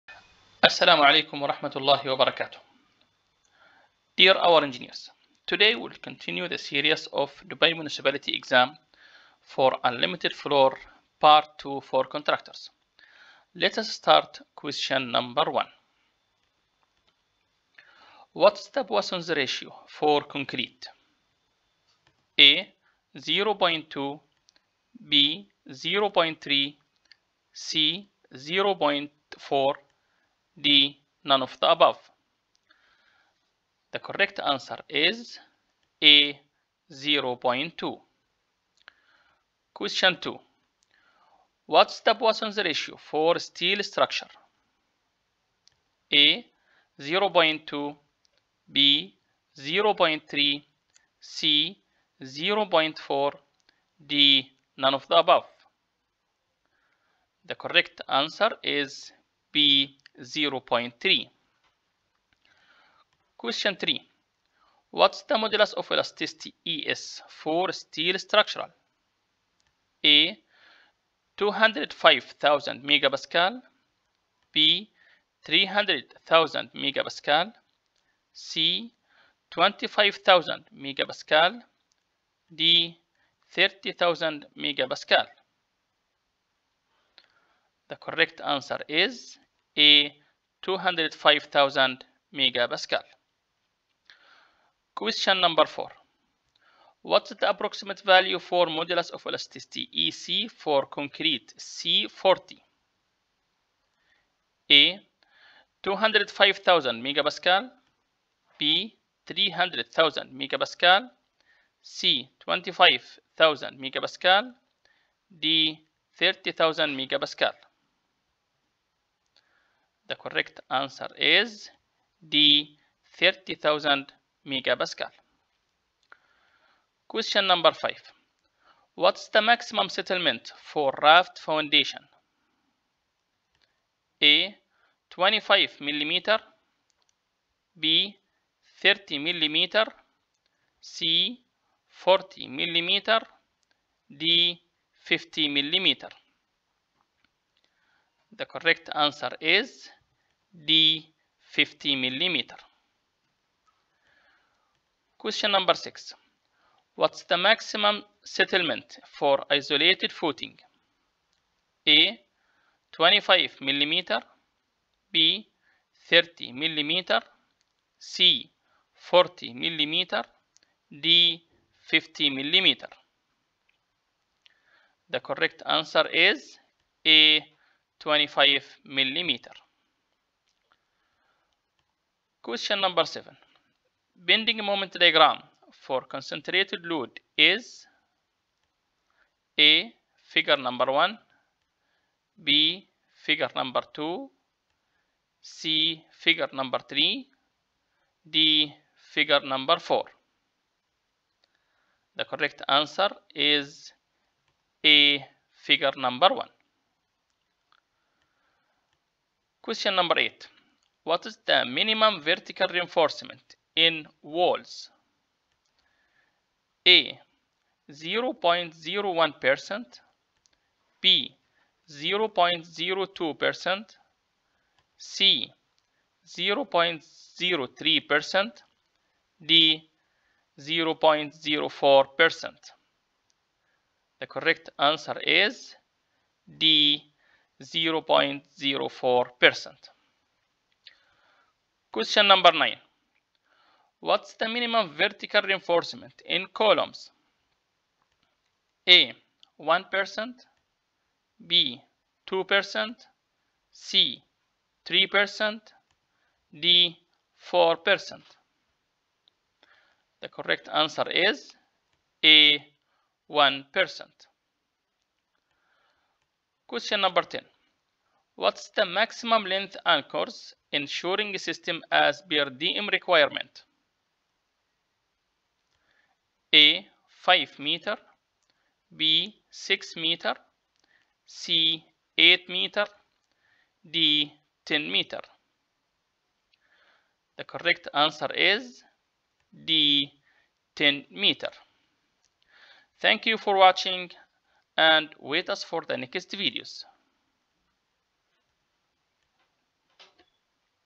Dubai Municipality Civil Engineer Exam 2022 for Unlimited Floors Part 2
Dubai Municipality Exam for civil engineers Unlimited Floors part 2-minimum 3 questions will come in the exam.
Dubai Municipality test questions and answers Unlimited Floors for Civil Engineers
Very important Interview questions for civil engineers in UAE
كيف تجتاز اختبار بلدية دبي
كيفية اجتياز امتحان بلدية دبي